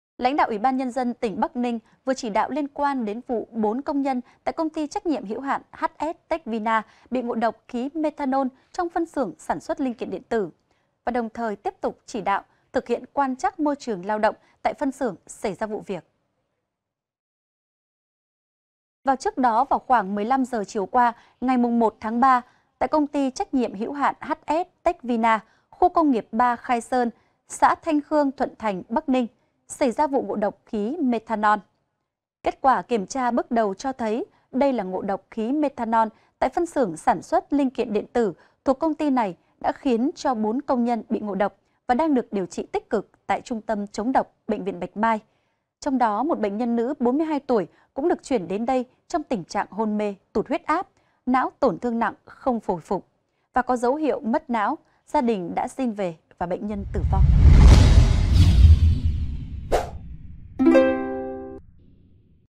Nhiều công nhân ngộ độc Methanol trong KCN ở Bắc Ninh | VTC14
VTC14 | Lãnh đạo UBND tỉnh Bắc Ninh vừa chỉ đạo khẩn liên quan đến vụ 4 công nhân tại Công ty TNHH HS Tech Vina, bị ngộ độ khí Methanol trong phân xưởng sản xuất linh kiện điện tử. Đồng thời tiếp tục chỉ đạo thực hiện quan trắc môi trường lao động tại phân xưởng xảy ra vụ việc.